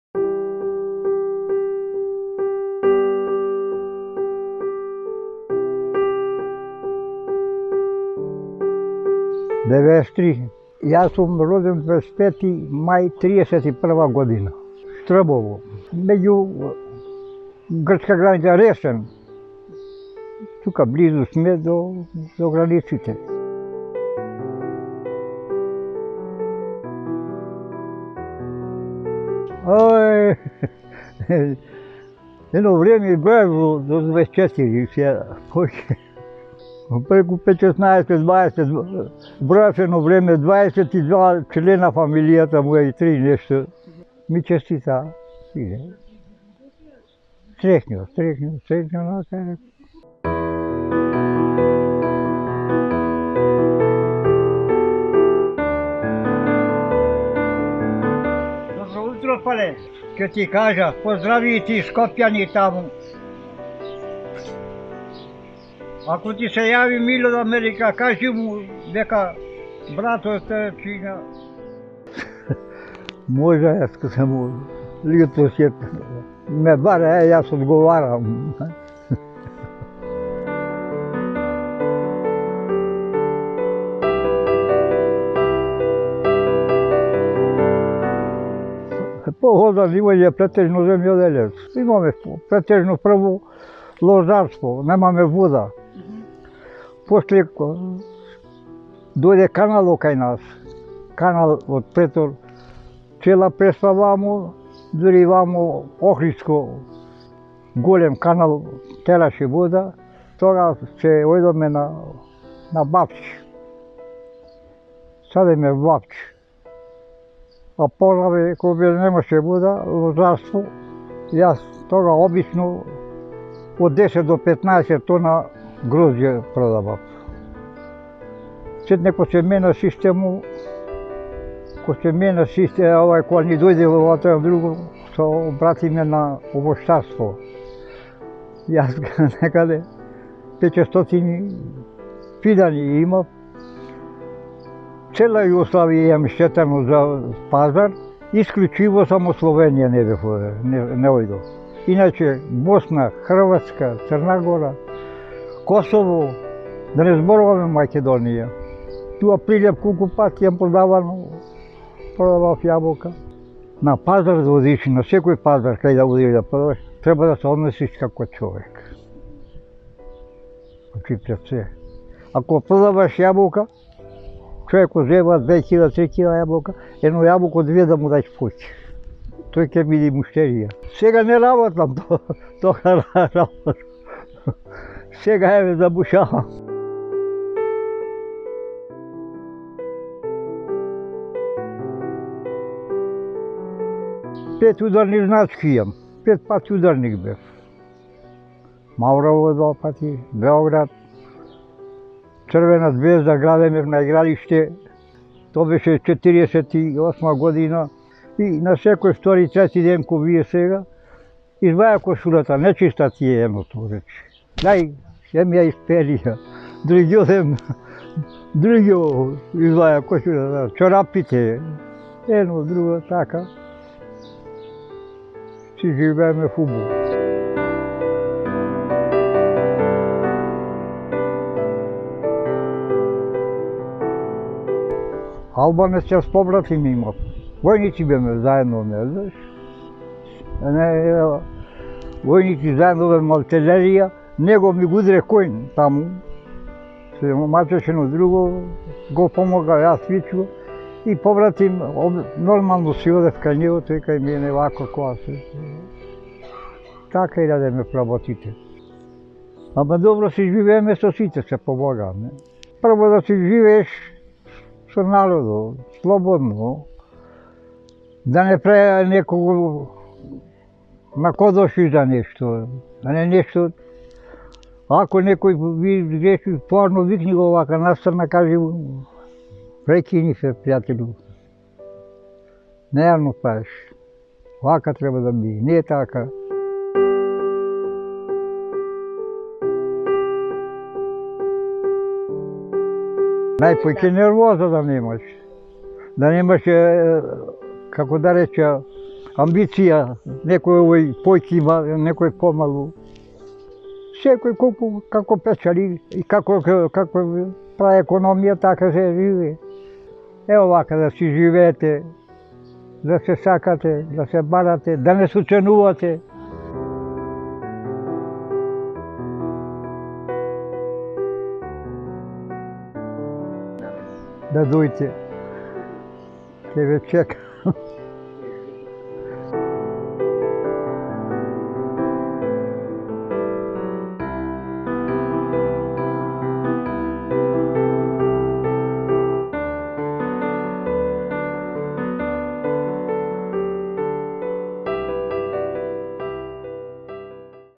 Приказна за долговечноста - за 93 годишниот Крсто Диневски роден на 25 мај
Крсто Диневски е најстариот станар во прилепскиот дома за стари лица.